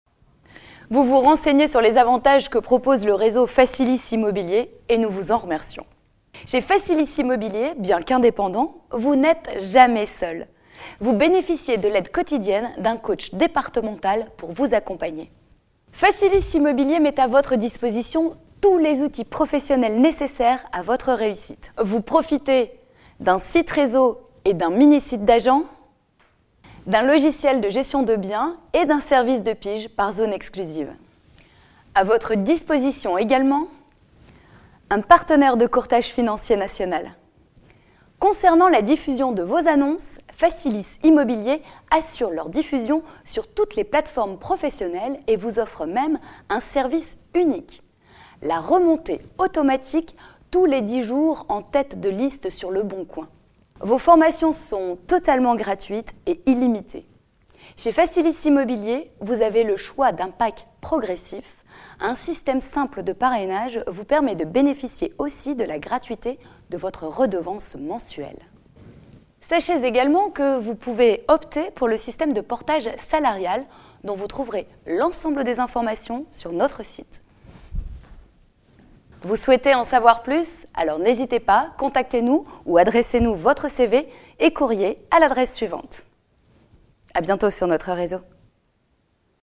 FACILIS PRESENTATION DU RESEAU
Vous vous renseignez sur les avantages que propose le réseau FACILIS IMMOBILIER et nous vous en remercions.
Chez FACILIS IMMOBILIER , bien qu’indépendant vous n’etes jamais seul ! Vous bénéficiez de l’aide quotidienne d’un coach départemental pour vous accompagner.

FACILIS IMMOBILIER met à votre disposition tous les outils professionnels nécessaires à votre réussite :
Vous profitez : d’un site réseau et un mini site d’agent, , d’un logiciel de gestion de biens et d’un service de pige par zone exclusive,
A votre disposition également : un partenaire de courtage financier national !

Concernant la diffusion de vos annonces, FACILIS IMMOBILIER assure leur diffusion sur toutes les plateformes professionnelles et vous offre même, un service unique :
La remontées automatique tous les 10 jours en tête de liste sur LEBONCOIN

Vos formations sont totalement gratuites et illimitées…

Chez FACILIS IMMOBILIER, vous avez le choix d’un pack progressif

Un système simple de parrainage vous permet de bénéficier aussi de la gratuité de votre redevance mensuelle
Sachez également que vous pouvez opter pour le système de portage salarial dont vous trouverez l’ensemble des informations sur notre site

Vous souhaitez en savoir plus ? N’hésitez pas, contactez-nous !
A bientôt sur notre réseau !

"Aidez Sofian dans son combat contre l'ataxie telangiectasie"